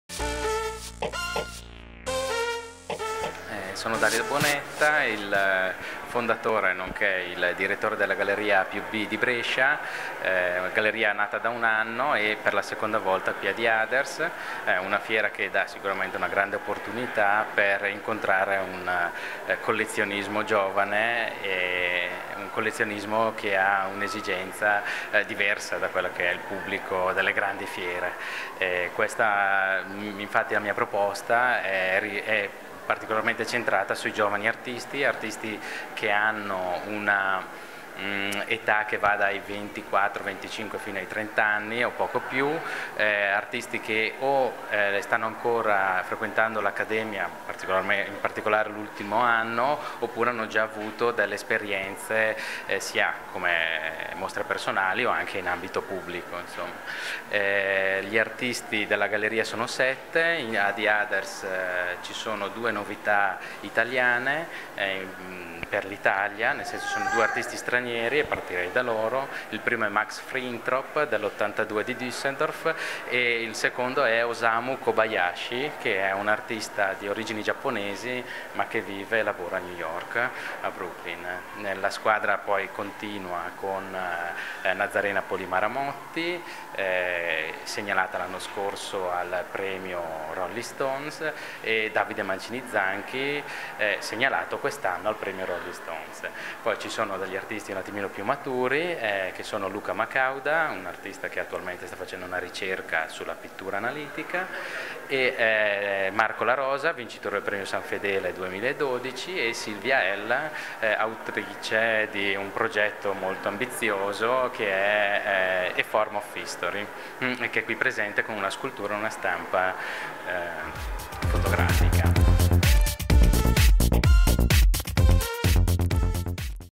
The Others - AplusB contemporary art (Brescia)
The Others
9 - 11 Novembre 2012 - Carceri Le Nuove, Torino.

Intervista a Dario Bonetta per AplusB contemporary art (Brescia).